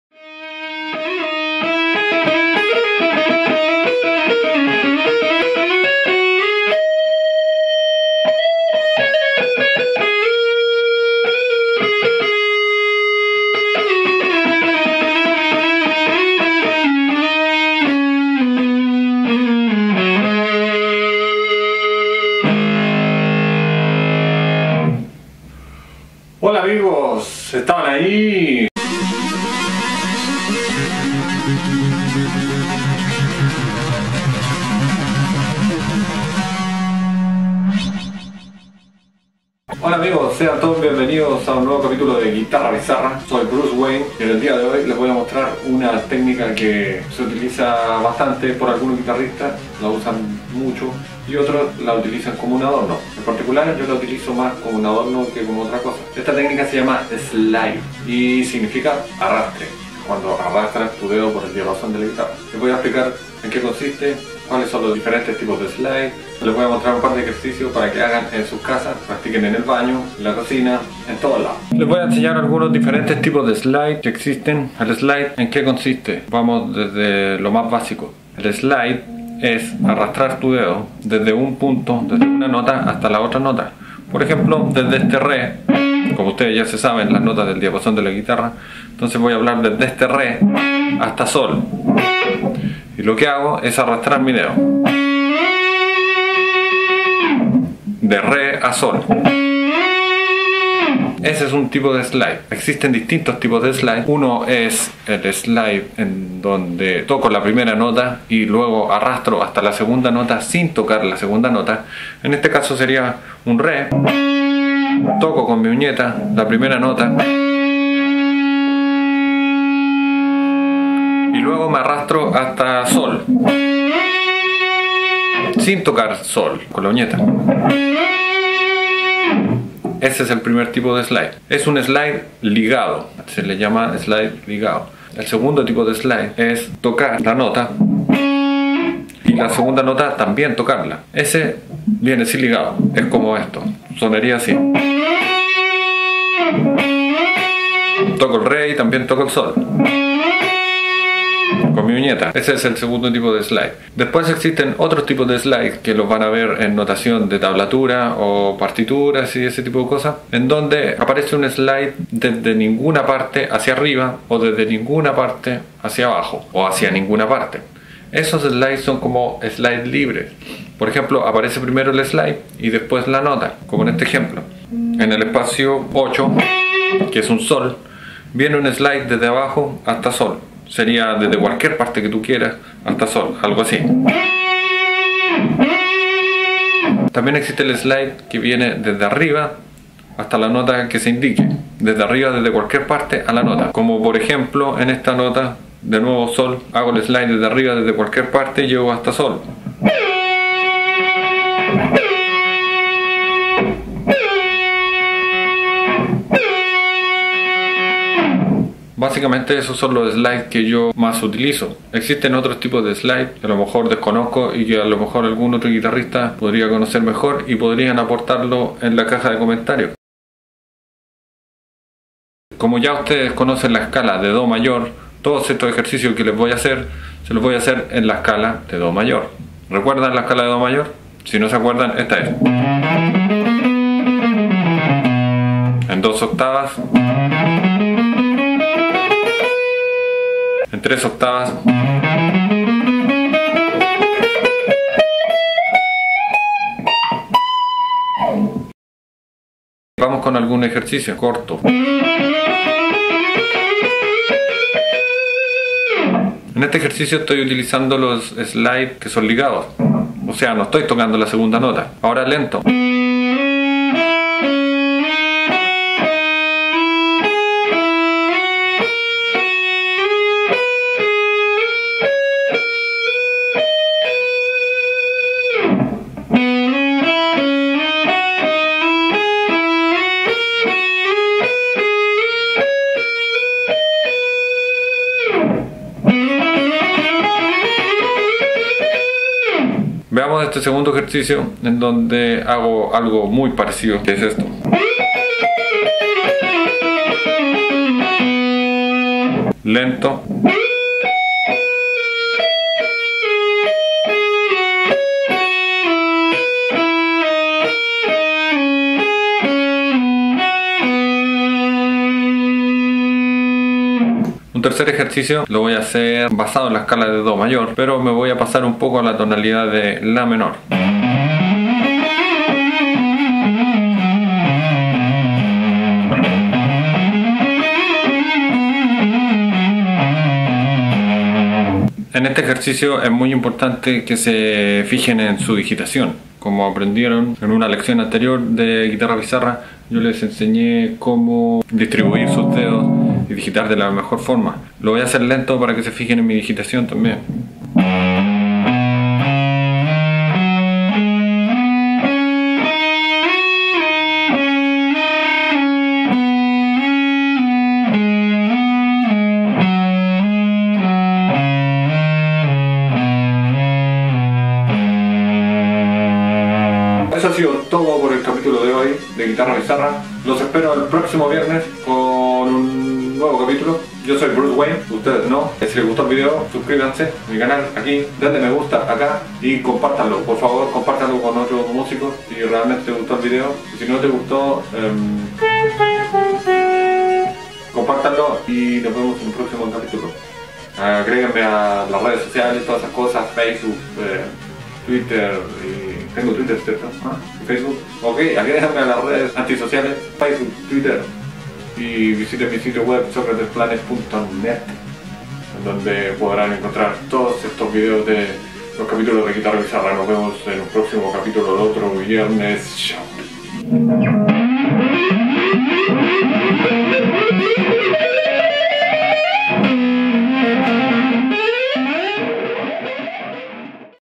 🤐🤐 Lo que NADIE te ha CONTADO sobre el SLIDE (GLISSANDO) en GUITARRA 🍾🍾 (Guitarra Bizarra / cap10)
En el capítulo 10 Sócrates el Guitarrista de la tercera luna de Júpiter nos muestra sus ancestrales poderes denominados Slide (in inglich) o Glissando (en españolo).